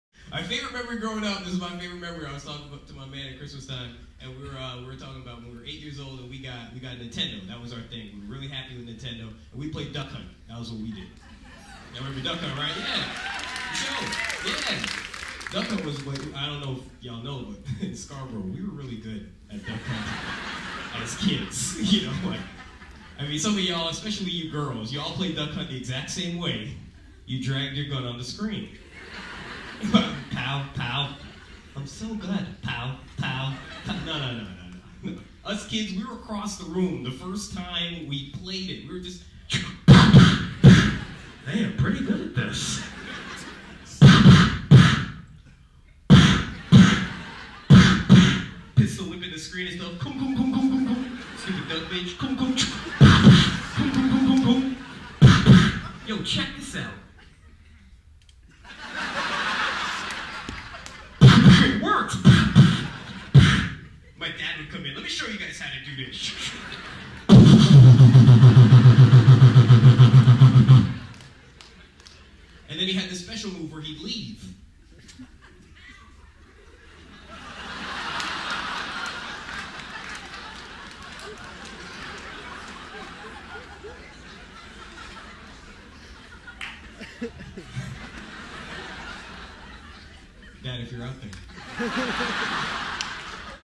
Quinn C. Martin on Video Games
Jamaican Canadian Comedian, Quinn C. Martin performing at The Most RACES Show on Earth! 2007.